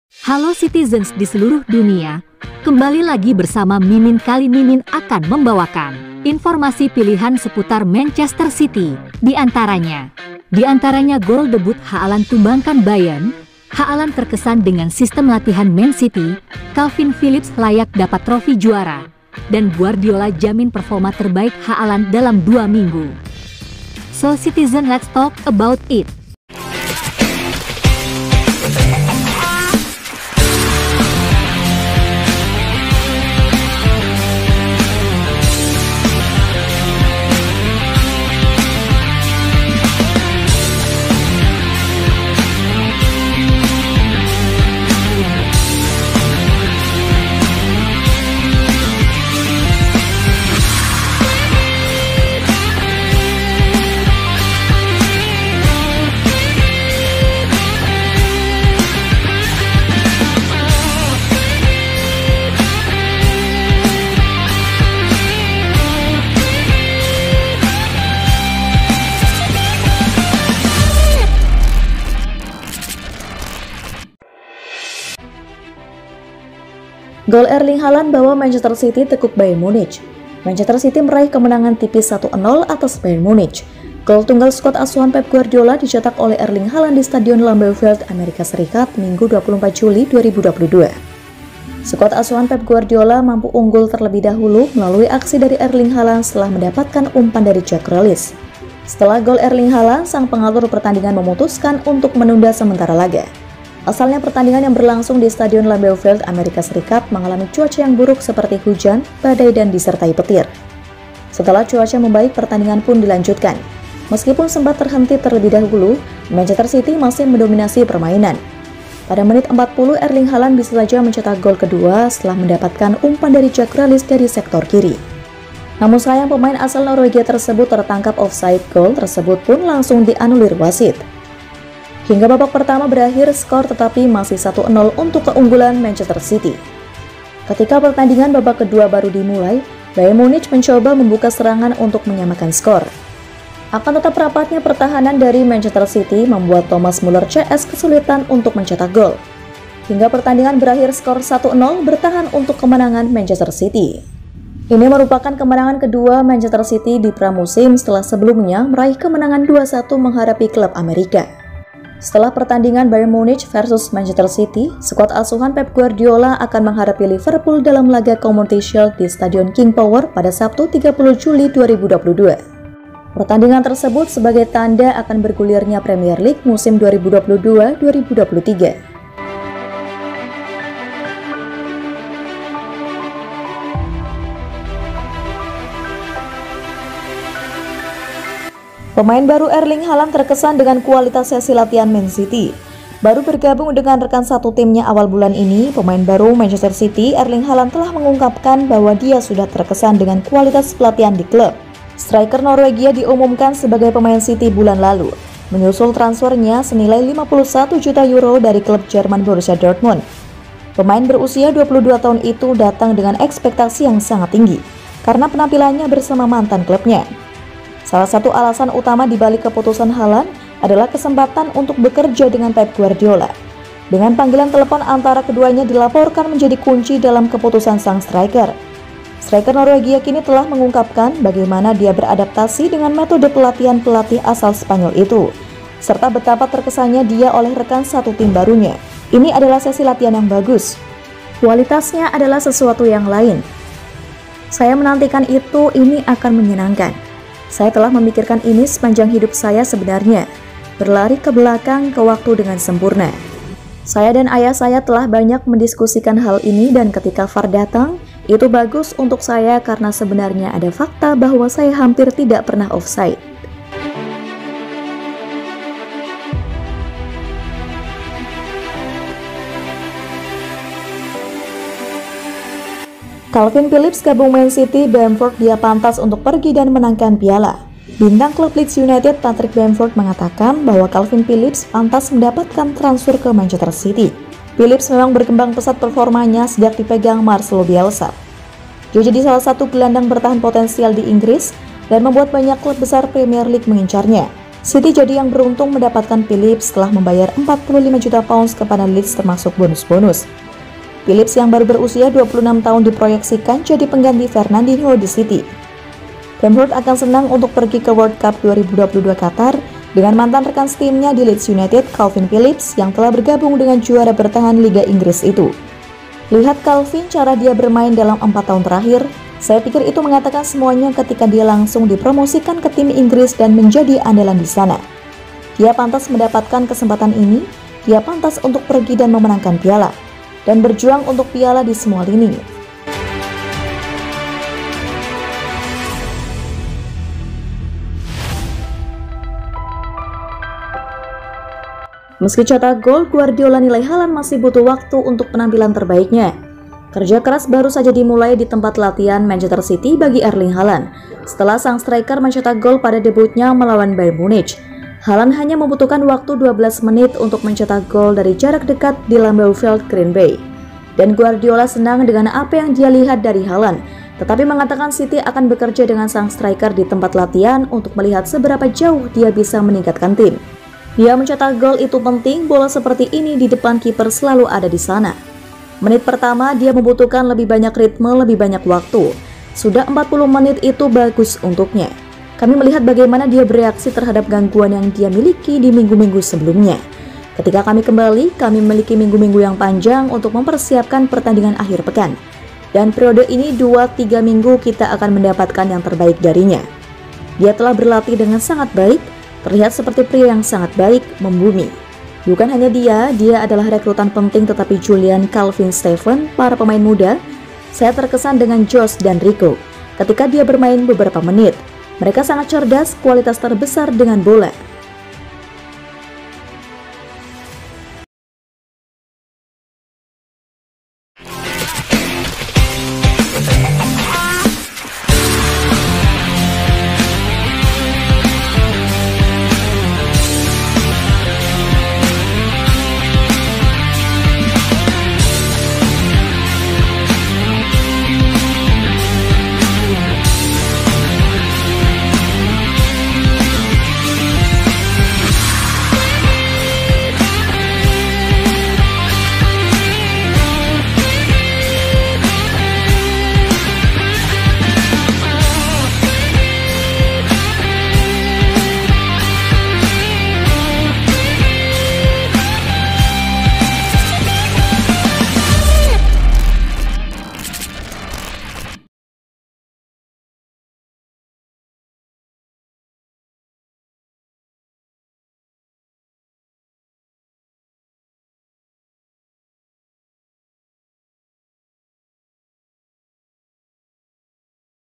LIHATLAH😍Kualitas Haaland Bikin Semua Terkejut😱Haaland Bisa Lebih Baik Lagi🔥Phillips Layak Ke City🏆
Sumber:
Luar: Manchester Evening News,mancity.com, Sky Sport, Goal.com, Daily Mail, Metro.co.uk, Caughtoffside, football.london.